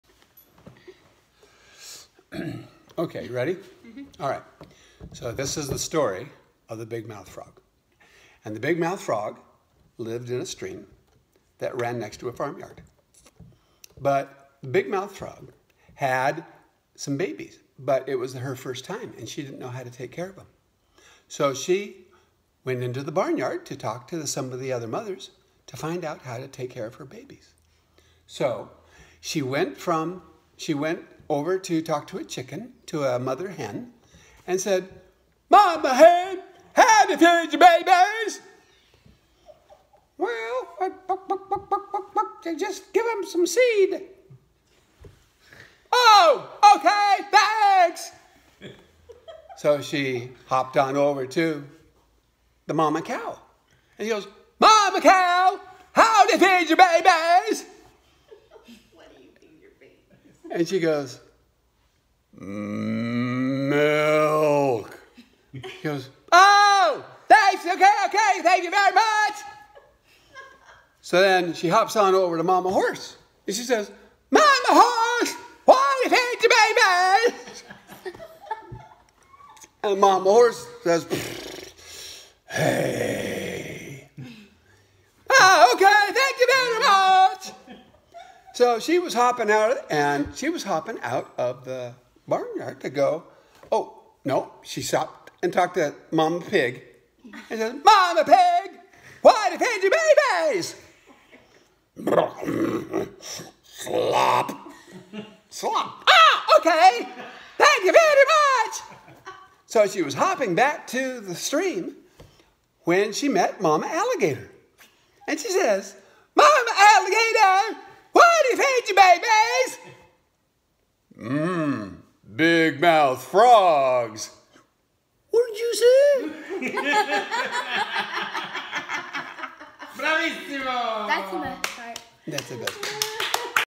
Story of the Big Mouth Frog 2018
Papa telling the story of the Big Mouth Frog